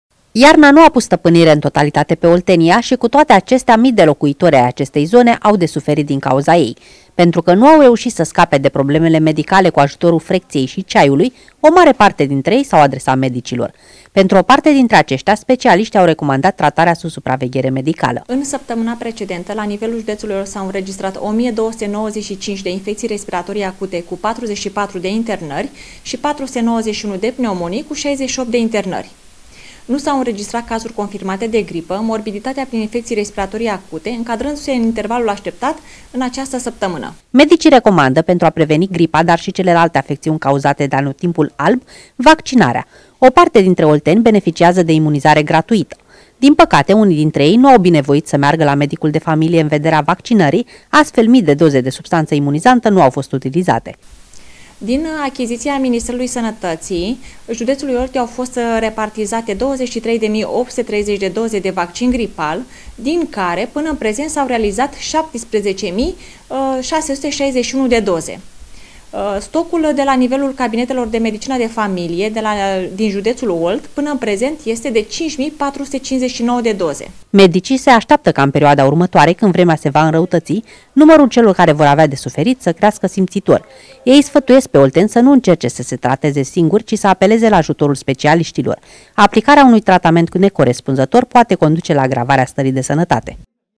Mii de olteni, cu sănătatea în ,,piuneze,, din cauza vremii
Numărul oltenilor diagnosticați cu afecțiuni de sezon se menține constant, peste 1750 de persoane apelând la ajutorul medicilor pentru a face față bolii. Cei mai mulți dintre cei care au trecut pragul cabinetelor medicale sunt tineri și au fost diagnosticați cu viroze respiratorii. Din fericire, până acum, niciun oltean nu a fost diagnosticat cu gripă. Mii de olteni care beneficiază de vaccin antigripal gratuit nu s-au prezentat la medicii de familie pentru imunizare.